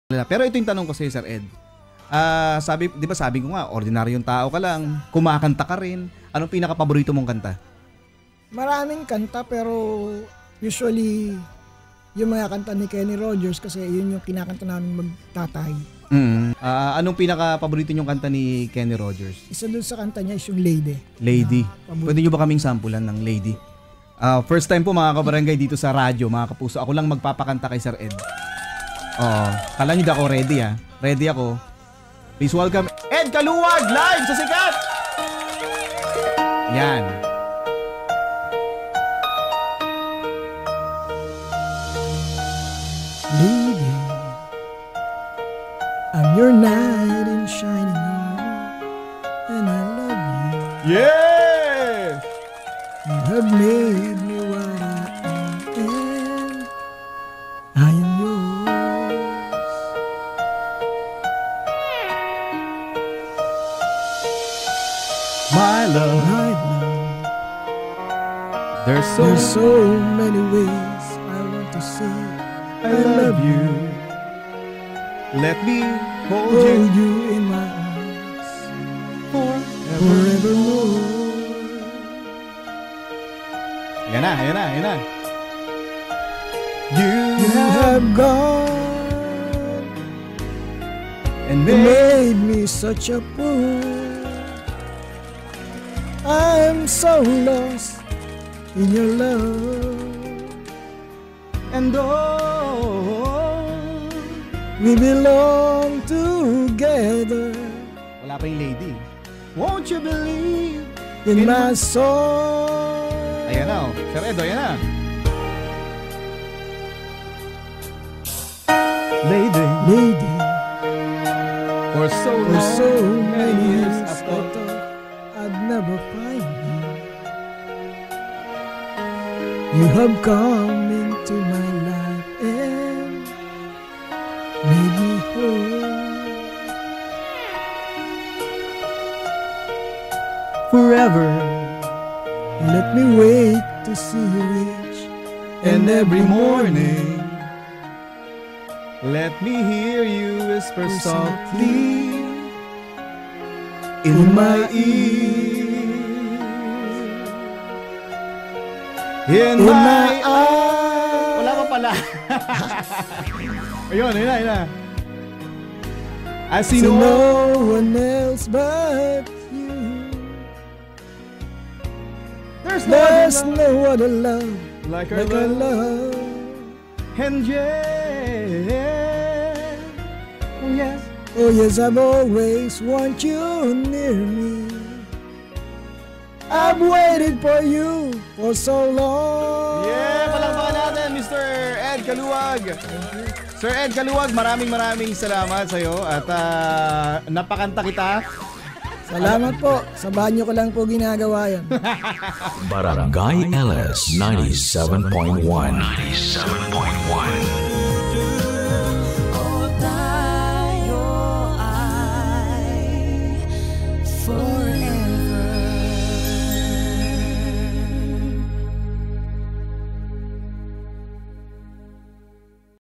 SIKAT: Ed Caluag, ipinamalas ang galing sa pag-awit!
Hindi lamang magaling sa pag-arte at pagiging paranormal expert si Ed Caluag, marunong din siyang umawit!